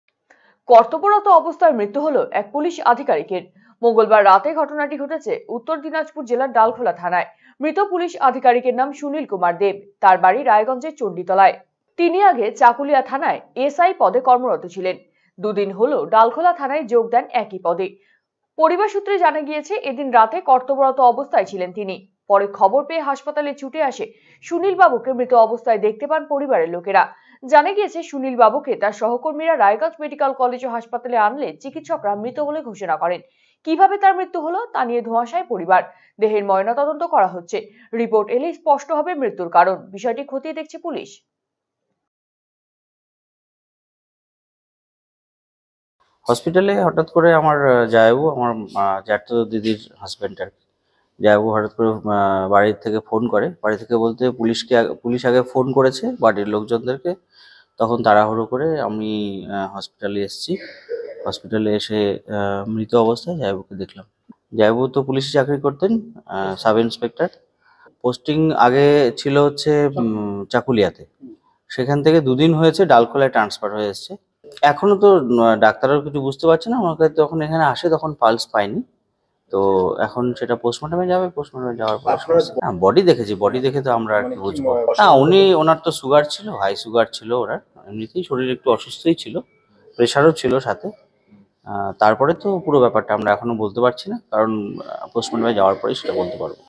Raiganj News | কর্তব্যরত অবস্থায় মৃত এক পুলিশ আধিকারিক | Rctv Sangbad
সংক্ষেপে খবরের বিষয়ঃ  কর্তব্যরত অবস্থায় মৃত্যু হল এক পুলিশ আধিকারিকের। মঙ্গলবার রাতে ঘটনাটি ঘটেছে উত্তর দিনাজপুর জেলার ডালখোলা থানায়। মৃত পুলিশ আধিকারিকের নাম সুনীল কুমার দেব। তার বাড়ি রায়গঞ্জের চন্ডীতলায়। তিনি আগে চাকুলিয়া থানায় এস আই পদে কর্মরত ছিলেন। ২ দিন হল ডালখোলা থানায় যোগ দেন একই পদে।